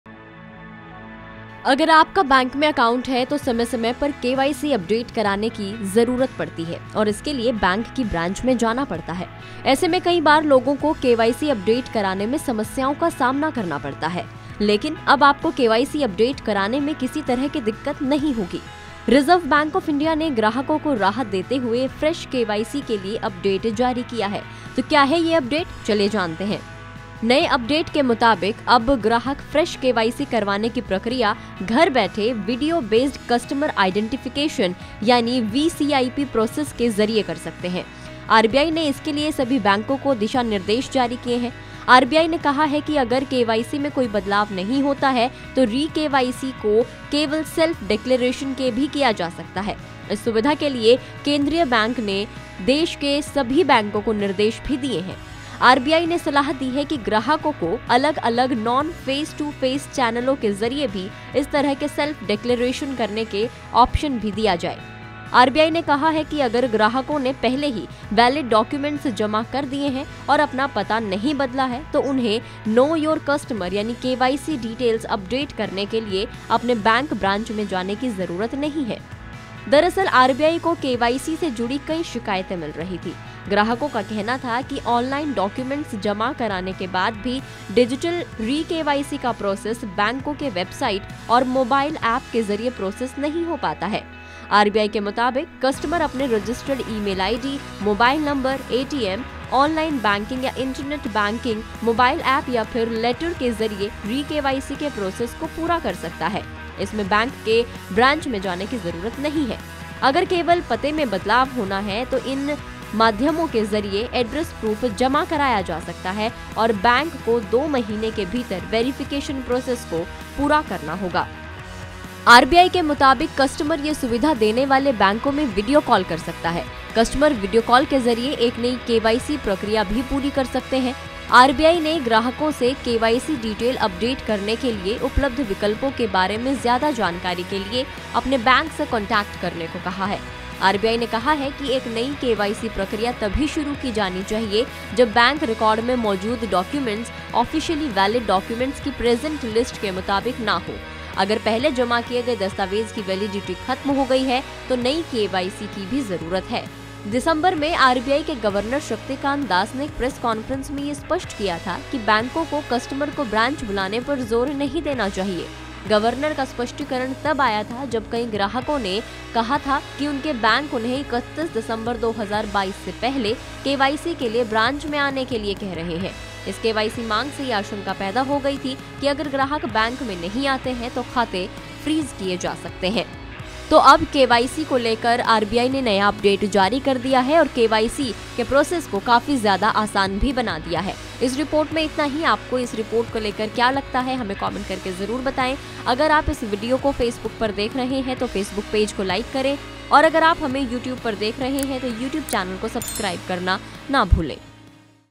RBI KYC Update: RBI ने KYC प्रोसेस को किया आसान, अब मिनटों में घर बैठे होगा काम | GoodReturns
अमूमन ग्राहकों को kyc update कराने के लिए बैंक की ब्रांच में जाना पड़ता है. ऐसे में कई बार ग्राहकों को समस्याओं का सामना करना पड़ता है. लेकिन अब  KYC अपडेट कराने में किसी तरह की दिक्कत नहीं होगी. RBI ने ग्राहकों को राहत देते हुए फ्रेश केवाईसी के लिए अपडेट जारी किया है. तो क्या है ये अपडेट, चलिए जानते हैं-